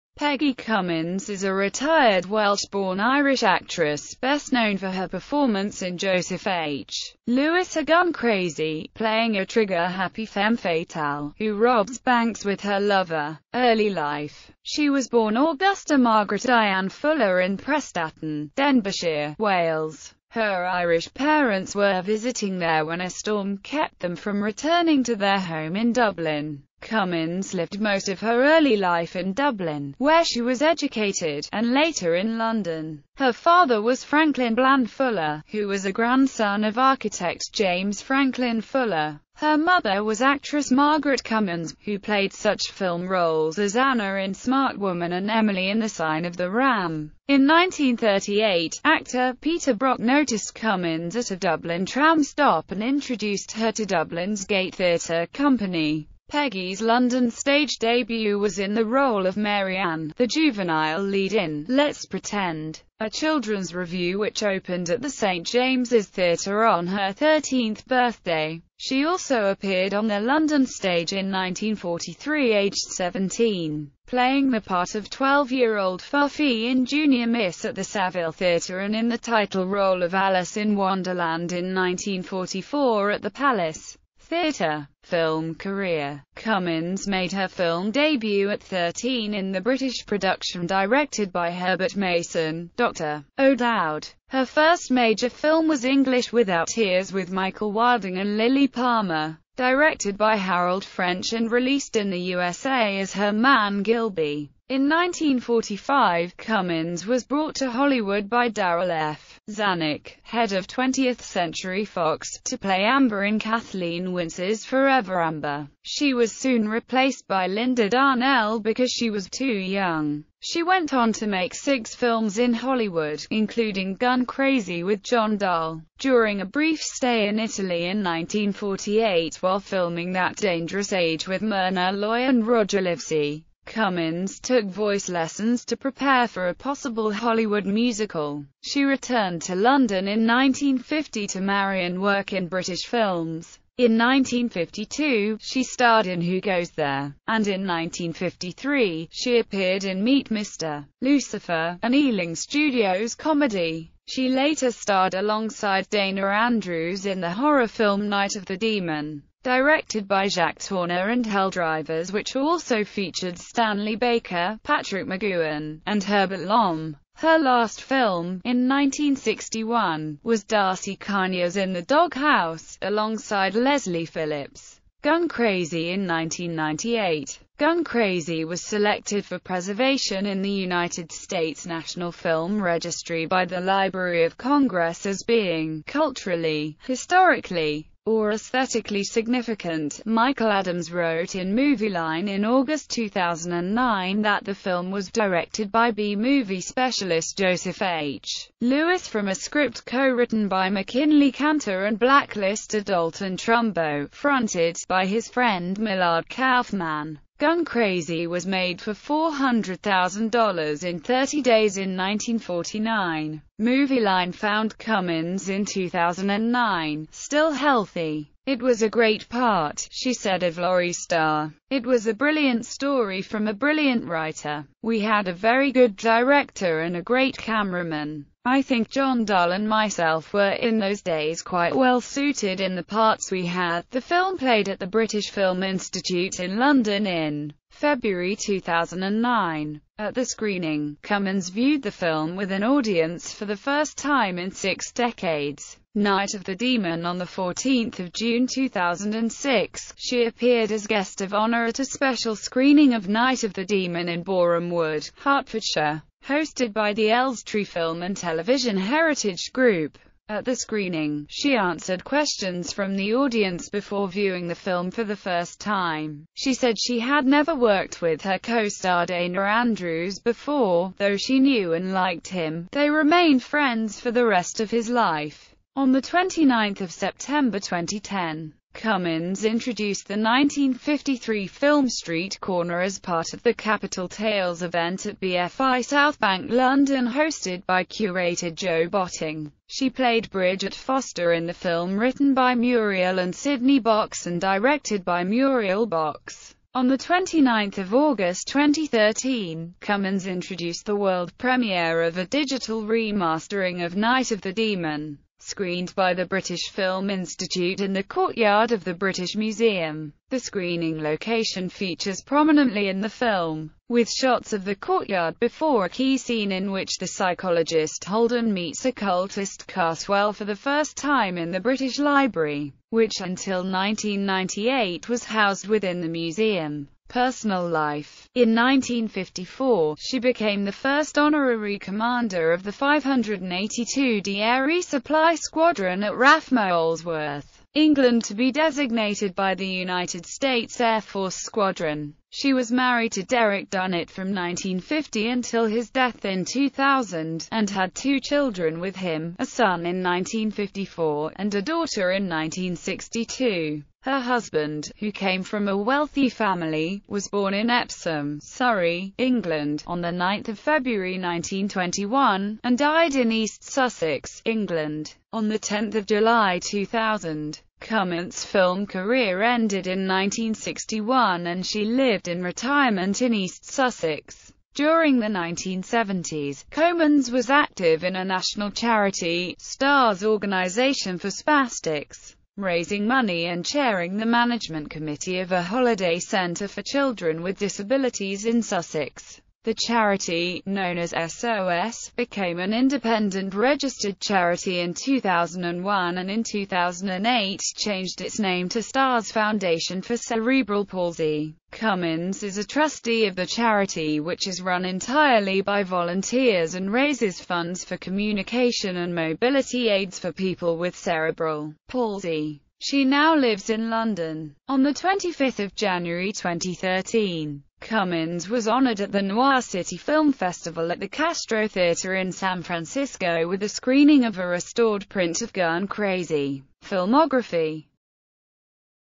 Peggy Cummins
Peggy Cummins (born 18 December 1925) is a retired Welsh-born Irish actress, best known for her performance in Joseph H.Lewis' Gun Crazy (1949), playing a trigger-happy femme fatale, who robs banks with her lover (played by John Dall).